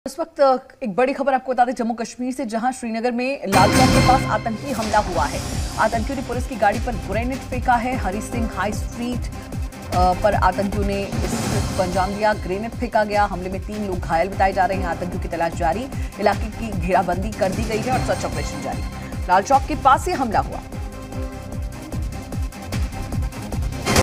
Breaking News: Srinagar के लाल चौंक पर आतंकी हमला | Jammu Kashmir | Latest News
Srinagar के Lal Chowk पर आतंकियों ने Police की गाड़ी पर Grenade फेंककर हमला किया है. हमले में तीन लोग घायल बताए जा रहे हैं.

About Channel